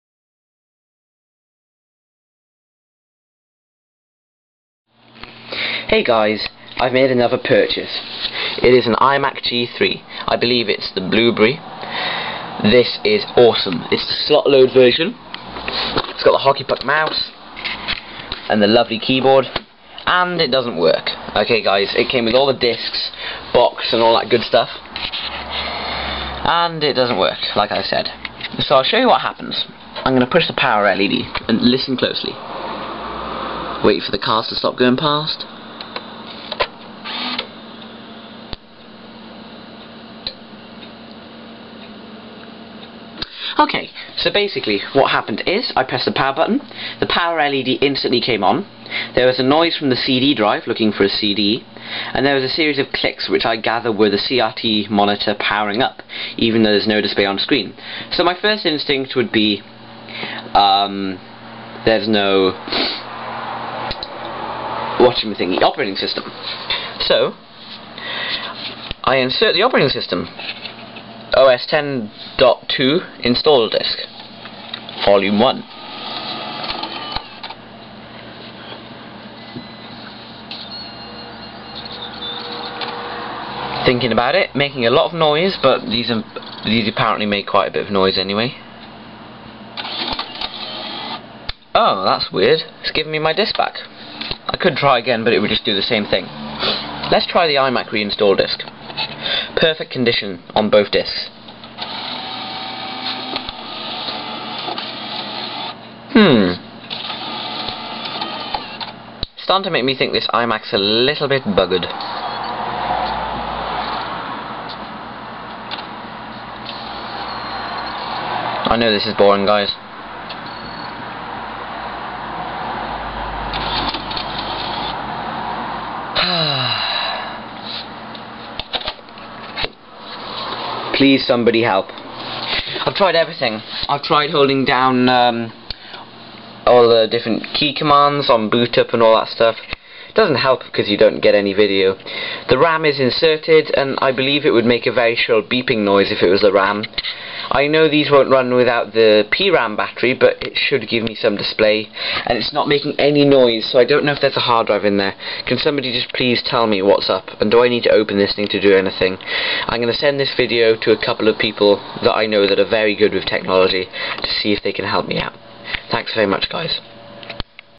Please Help Me! iMac G3
Just a very quick video.

This iMac is a lovely computer and I really like it. I just wish that it was working!
I have nothing much to say apart from, check out this video and please let me know if you have any idea as to what might be wrong with this iMac G3! :(